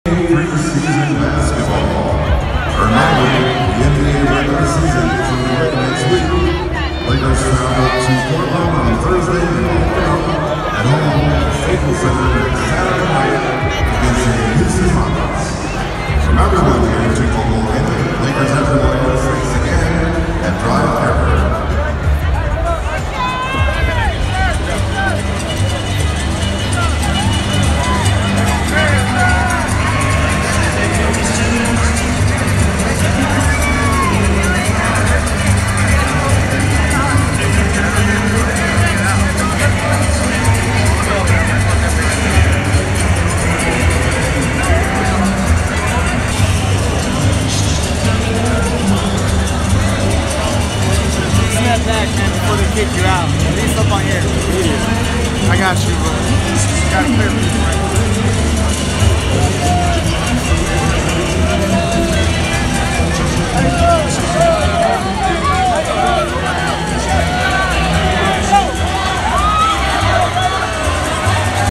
Postgame views: Jordan Bell and DeMarcus "Boogie" Cousins after Lakers from Vegas, NV
From postgame Warriors vs Lakers (4th preseason game) in Las Vegas, NV at T-Mobile Arena.